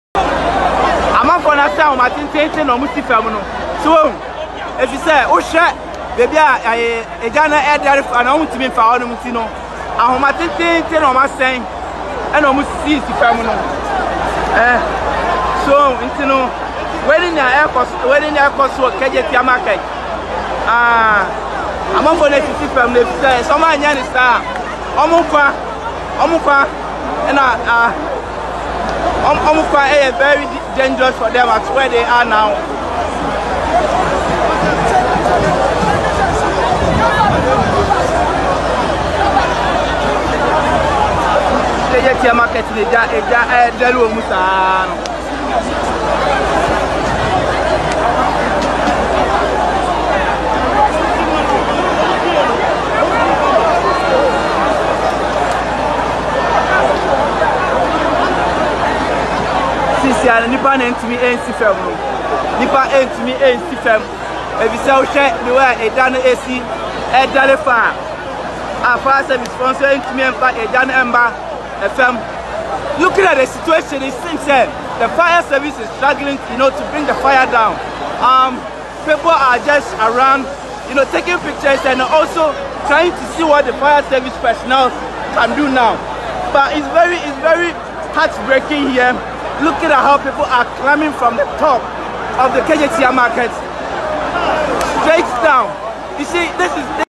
Here is how some victims escaped from the fire outbreak at the recently constructed Kejetia market.
Fire gutted parts of the recently constructed Kejetia market in Kumasi.
Here is how some victims escaped from the fire.